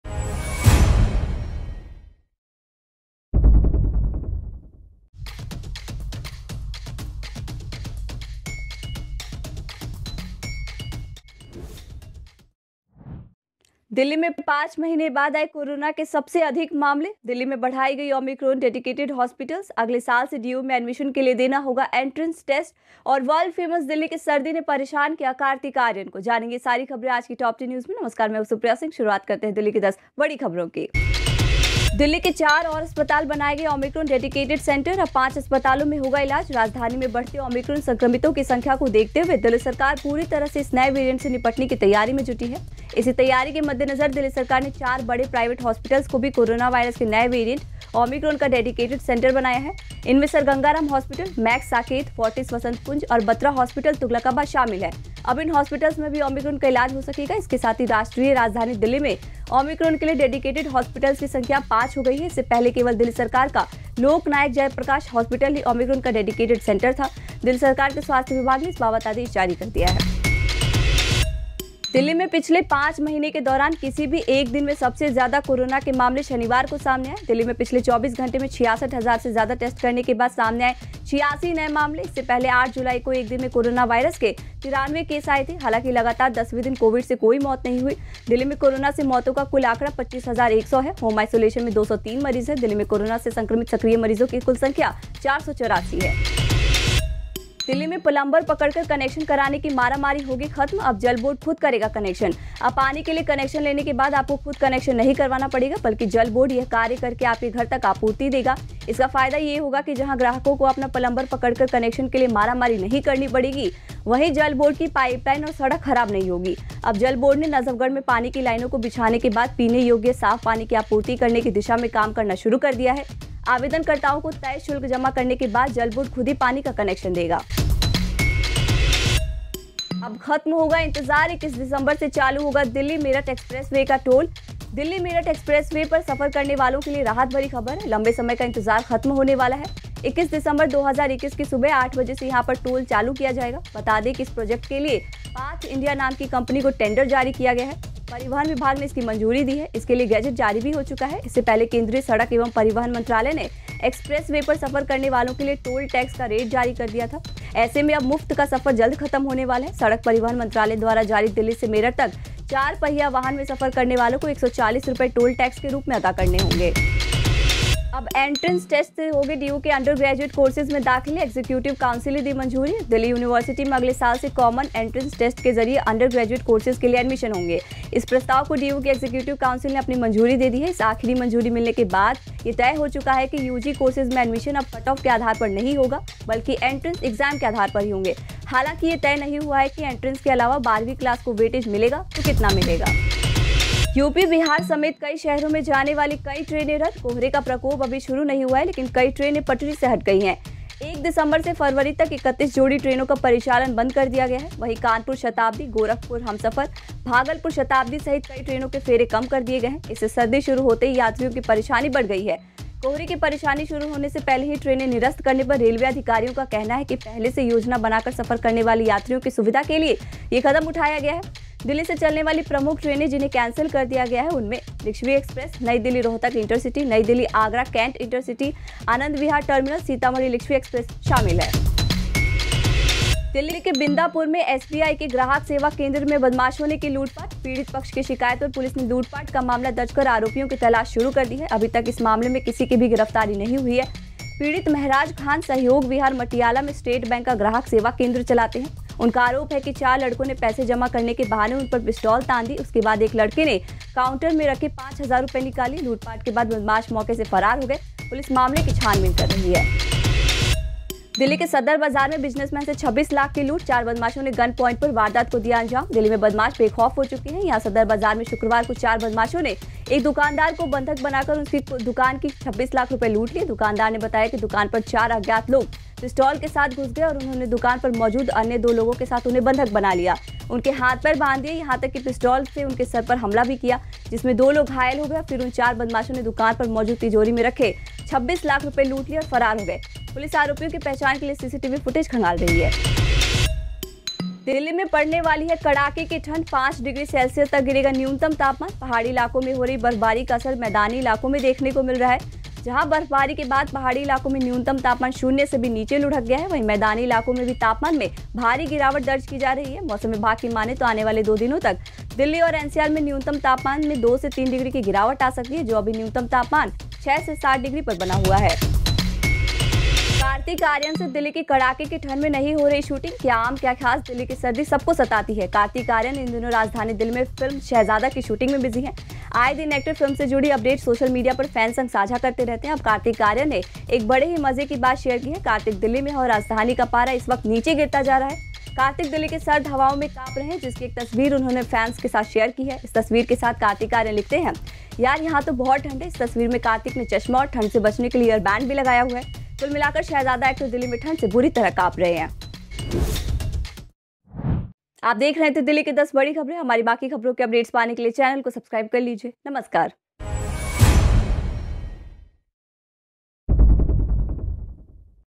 19 December 2021 | Delhi Top Ten News | दिल्ली की आज की 10 बड़ी खबरें | रोज देखें और Update रहें
19 December 2021 | Delhi Top Ten News | दिल्ली की आज की 10 बड़ी खबरें | रोज देखें और Update रहें | Kejriwal | DJB | Water Connection | Loot | fraud